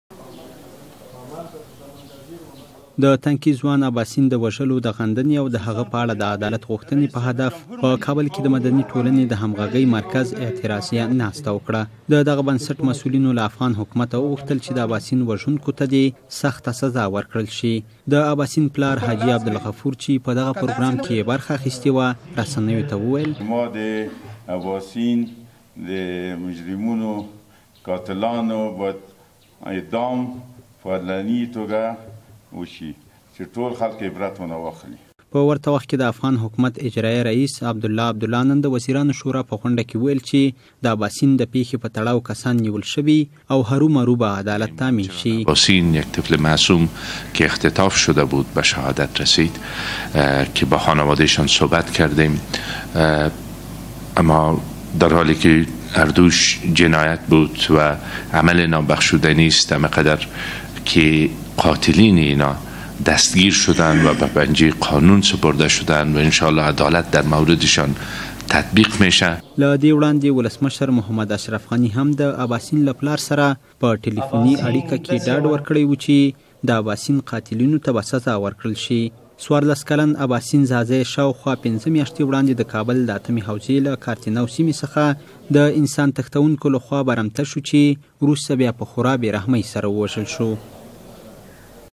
‘د اباسین وژونکو به هرو مرو سزا وینی’
په کابل کې د ځوان اباسین وژلو د غندنې او د هغه په اړه د عدالت غوښتنې په هدف مدني ټولنې د همغږۍ مرکز اعتراضیه ناسته وکړه.

 په ورته وخت کې د افغان حکومت اجرائیه رئیس عبدالله عبدالله د سېشنبې په ورځ د وزیرانو شورا په غونډه کې وویل چې د اباسین د پېښې په تړاو کسان